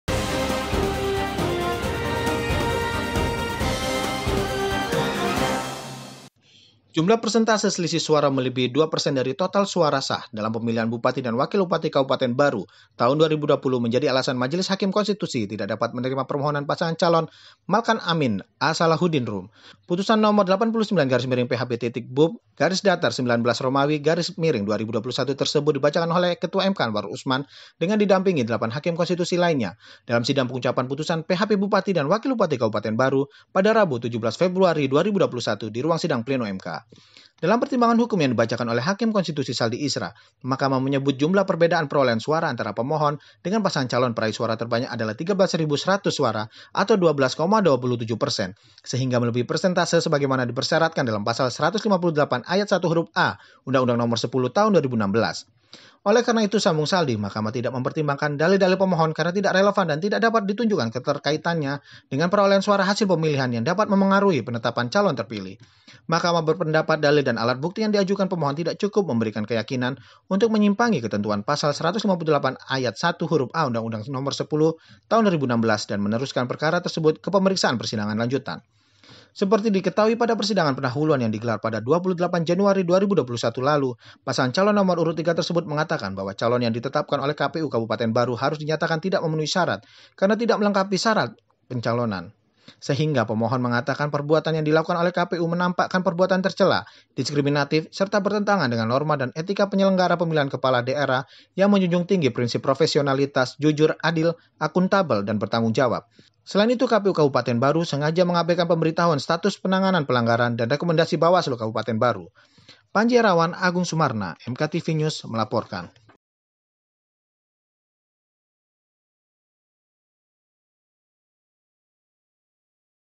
Selisih Suara Lebihi Ambang Batas, Permohonan Malkan Amin – A. Salahuddin Rum Kandas
JAKARTA, HUMAS MKRI – Jumlah persentase selisih suara melebihi 2% dari total suara sah dalam  Pemilihan Bupati dan Wakil Bupati Kabupaten Barru Tahun 2020 menjadi alasan Majelis Hakim Konstitusi tidak dapat menerima permohonan Pasangan Calon Malkan Amin – A. Salahuddin Rum. Putusan Nomor 89/PHP.BUP-XIX/2021 tersebut dibacakan oleh Ketua MK Anwar Usman dengan didampingi delapan hakim konstitusi lainnya dalam sidang pengucapan Putusan PHP Bupati dan Wakil Bupati Kabupaten Barru, pada Rabu (17/2/2021) siang di Ruang Sidang Pleno MK.

Dalam pertimbangan hukum yang dibacakan oleh Hakim Konstitusi Saldi Isra, Mahkamah menyebut jumlah perbedaan perolehan suara antara Pemohon dengan pasangan calon peraih suara terbanyak adalah paling banyak 2% dari total suara sah atau 2% dari 105.969 suara atau sejumlah 2.119 suara.

“Perolehan suara Pemohon adalah 35.964 suara, sedangkan perolehan suara pihak terkait adalah 49.064 suara, sehingga selisih perolehan suara antara Pemohon dan pihak terkait adalah 13.100 suara (12,27%) sehingga melebihi persentase sebagaimana dipersyaratkan dalam Pasal 158 ayat (1) huruf a UU Nomor 10 Tahun 2016,” ujar Saldi membacakan Putusan Nomor 89/PHP.BUP-XIX/2021 tersebut.

Oleh karena itu, sambung Saldi, Mahkamah tidak mempertimbangkan dalil-dalil Pemohon karena tidak relevan dan tidak dapat ditunjukkan keterkaitannya dengan perolehan suara hasil pemilihan yang dapat memengaruhi penetapan calon terpilih. Mahkamah berpendapat dalil dan alat bukti yang diajukan Pemohon tidak cukup memberikan keyakinan untuk menyimpangi ketentuan Pasal 158 ayat (1) huruf a UU 10/2016 dan meneruskan perkara a quo ke pemeriksaan persidangan lanjutan.

“Mahkamah berpendapat, meskipun permohonan yang diajukan Pemohon adalah kewenangan Mahkamah; permohonan diajukan masih dalam tenggang waktu pengajuan permohonan; dan Pemohon adalah Pasangan Calon Bupati dan Wakil Bupati Kabupaten Barru dalam Pemilihan Bupati dan Wakil Bupati Kabupaten Barru Tahun 2020, namun Pemohon tidak memenuhi ketentuan pengajuan permohonan sebagaimana dimaksud dalam Pasal 158 ayat (2) UU 10/2016. Dengan demikian, menurut Mahkamah eksepsi Termohon dan eksepsi Pihak Terkait bahwa Pemohon tidak memiliki kedudukan hukum adalah beralasan menurut hukum,” tegas Saldi.

Pada persidangan pendahuluan yang digelar pada 28 Januari 2021 lalu, Paslon Nomor Urut 3 tersebut mengatakan bahwa calon yang ditetapkan oleh KPU Kabupaten Barru harus dinyatakan tidak memenuhi syarat karena tidak melengkapi syarat pencalonan. Sehingga, Pemohon mengatakan, perbuatan yang dilakukan oleh KPU menampakkan perbuatan tercela, diskriminatif serta bertentangan dengan norma dan etika penyelenggara pemilihan kepala daerah yang menjunjung tinggi prinsip profesionalitas,  jujur, adil,  akuntabel dan bertanggung jawab. Selain itu, KPU Kabupaten Barru sengaja mengabaikan pemberitahuan status penanganan pelanggaran dan rekomendasi Bawaslu Kabupaten Barru. (*)